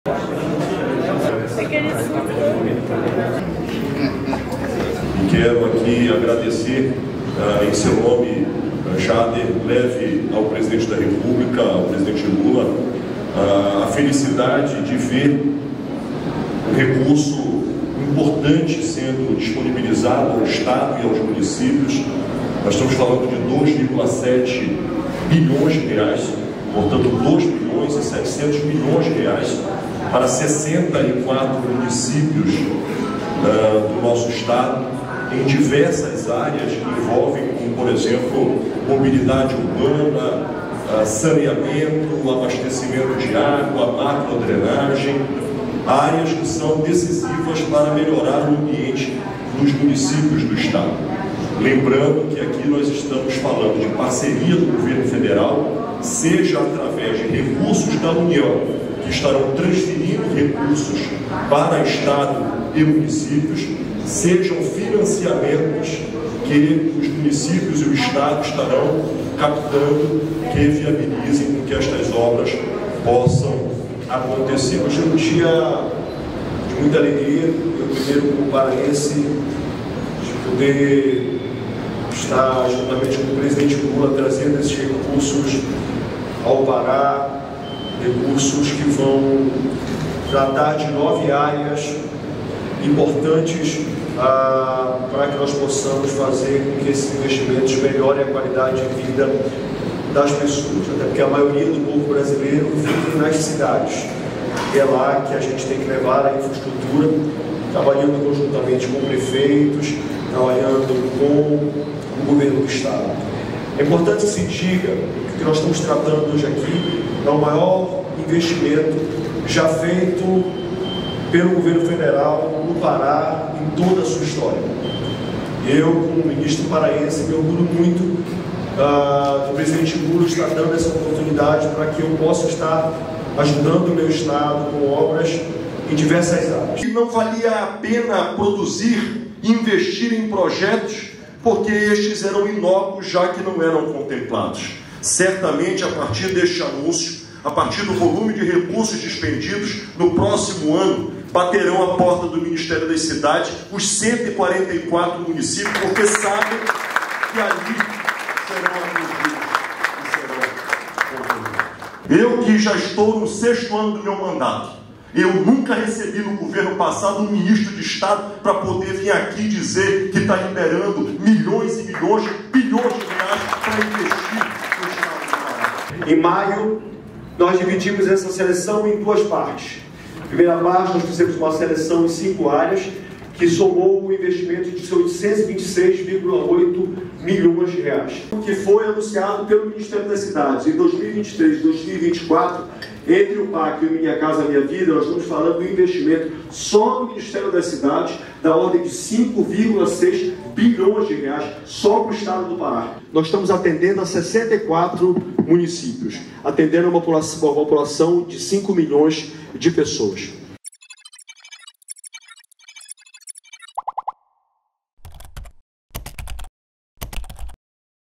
“Obras do Novo Pac Seleções devem gerar mais de 34 mil empregos”, aponta Governo Federal
O Governo Federal apresentou, nesta terça-feira (06), a ordem de 2,6 bilhões de reais do Novo PAC Seleções, que vai atender 112 propostas de 64 municípios paraenses. Segundo o ministro das Cidades, Jader Filho, esse é o maior investimento já realizado a nível federal para o Pará e deve gerar cerca de 34 mil empregos.

Reportagem: Kamila Murakami
Imagens: Igor Mota